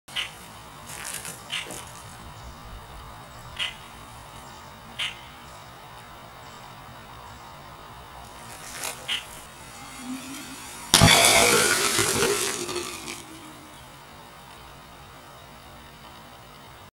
Brake fluid and Pool Shock (Calcium Hypochlorite)
This clip is at 1/2 original frame-rate.

Calcium Hypochlorite is not as quick to react with DOT-3 brake fluid as Sodium Hypochlorite:  a full minute of realtime footage was removed between the addition of fluid and its spontaneous combustion.  However, twice as much chlorate is available to the reaction per mole, and so much less hypochlorite powder is required for for similar results.   However, the generally longer yet unpredictable delay in initiation is undesirable, and riskier.